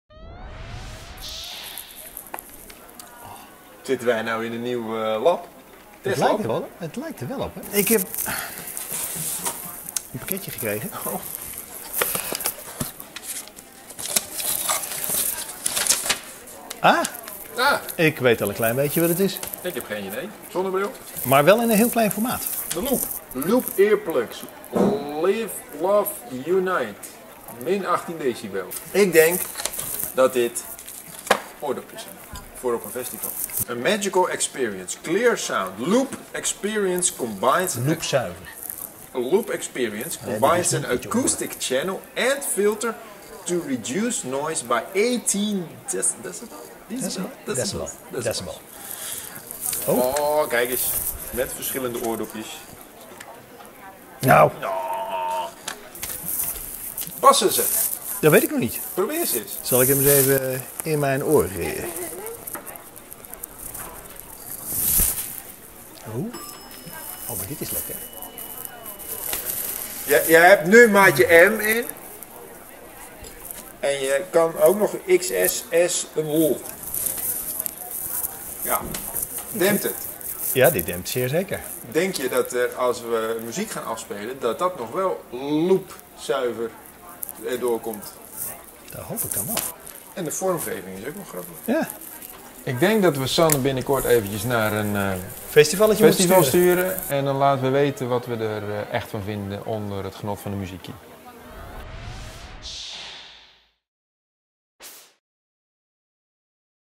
Loop earplugs
We houden wel van een feestje, maar ook van onze oren. Dus als we naar een concert gaan, doen we graag gehoorbescherming in. Het Belgische Loop maakte een variant geïnspireerd op Tomorrowland, die ze naar ons opstuurde. U vraagt, wij testen!

De oordoppen van Loop maken gebruik van filters die geluidsgolven niet helemaal blokkeren. Met deze filtertechnologie en het innovatieve design, biedt Loop volgens eigen zeggen hoogkwalitatieve en comfortabele gehoorbescherming zonder dat het geluid dof klinkt.

De Loop x Tomorrowland Experience Earplugs zijn een limited edition en voorzien van een gouden ring met een opdruk van de Tomorrowland-slogan ‘Live, Love, Unite’. Er wordt een bijpassend doosje meegeleverd.

In de verpakking vinden we vier verschillende maten dopjes om een goede pasvorm te hebben. En een goede pasvorm hebben ze. Sanne nam ze mee naar een concert en droeg ze de hele avond. Comfortabel dus. Sterker nog, hij zou er mee kunnen slapen, zegt ie.

Maar belangrijk is uiteraard ook de geluidservaring. En die is inderdaad niet dof. De lage tonen komen goed door, maar de hoge tonen worden wel merkbaar gefilterd. Dat haalt een klein beetje karakter van de muziek weg, maar ja, het beschermt wel tegen die schelle geluiden waar je later problemen mee kan krijgen. Een goed geslaagde gehoorbescherming dus, die er bovendien goed uitzien én erg lekker zitten. Op naar het volgende feestje!